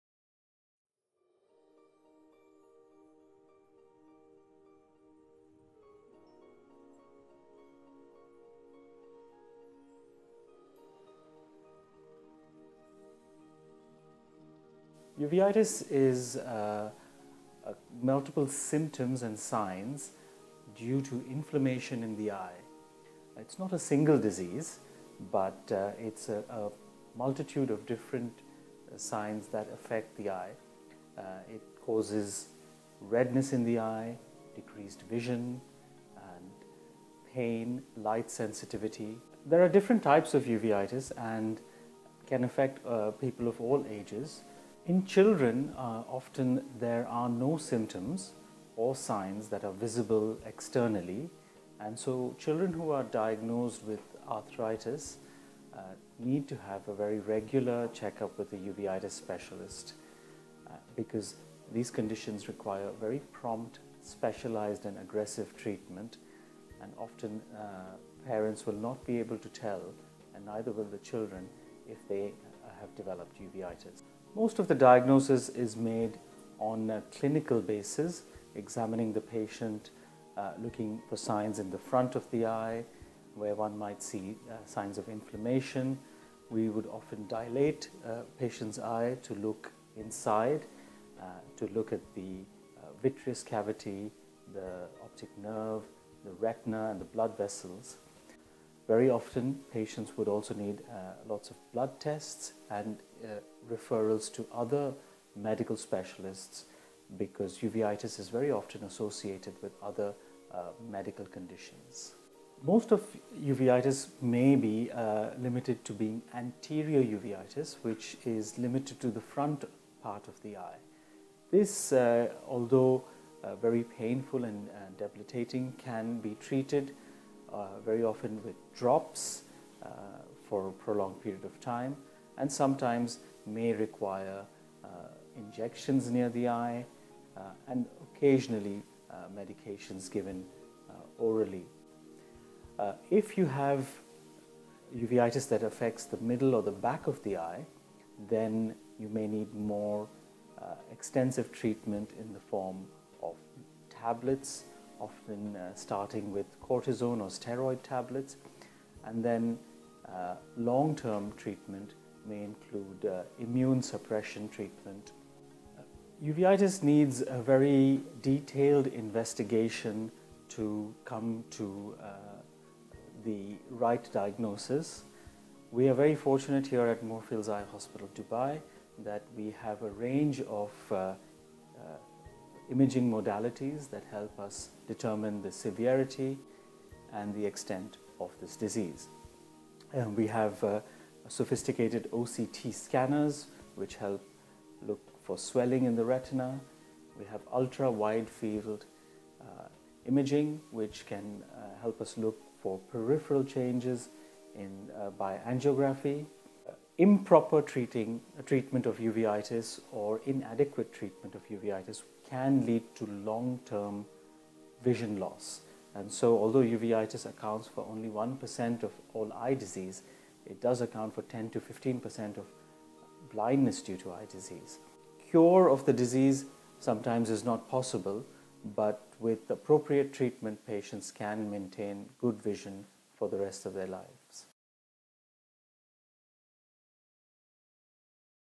Uveitis | Moorfields Eye Hospital Dubai
Uveitis is a clinical spectrum of symptoms and signs caused by a variety of medical conditions. It is caused by the inflammation of the middle layer of the eye, called the uvea or uveal tract. Uveitis causes 10-15% of blindness and needs to be managed carefully. The main treatment of Uveitis is steroid medication (corticosteroids), which reduces the inflammation inside the eye.

Here is Dr. Avinash Gurbaxani , Consultant Ophthalmic Surgeon in Uveitis and Medical Retina Diseases and Cataract Surgery at Moorfields Eye Hospital Dubai, talking about Uveitis.